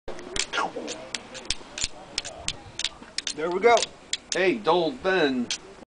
30second fire
an FCF Greenhorn named 30seconds builds a fire w/flintNsteel